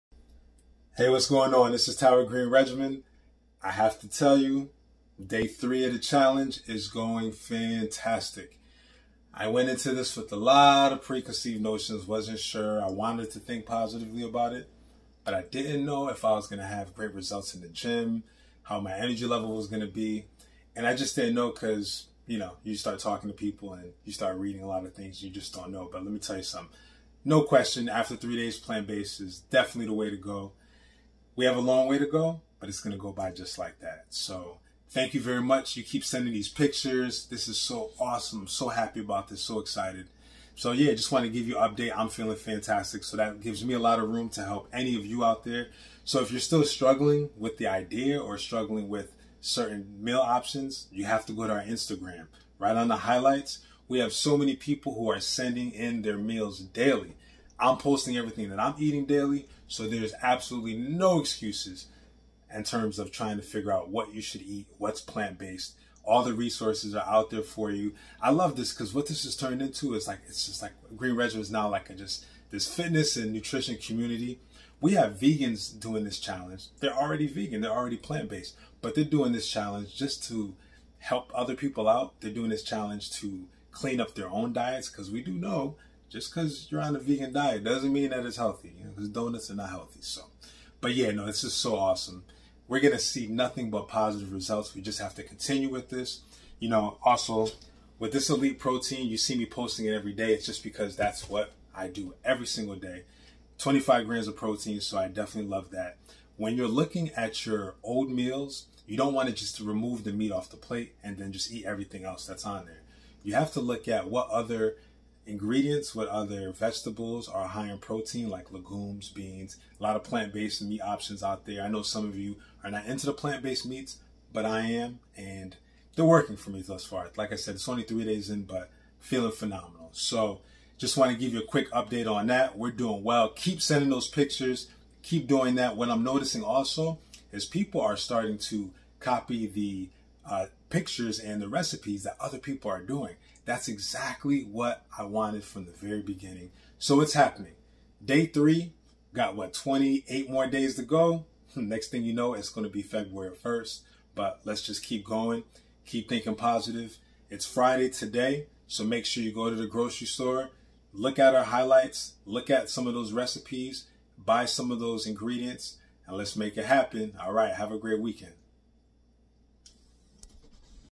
100% Plant-Based Challenge - Part 14 [ FOCUS DAY 3]
100% Plant-Based Challenge - Part  14 [ FOCUS DAY 3]
It's time to level up your lifestyle!
Green Regimen is going to conduct a 100% Healthy  Plant-Based Challenge.

When: January 1, 2020
Check our Instagram to learn more about our challenge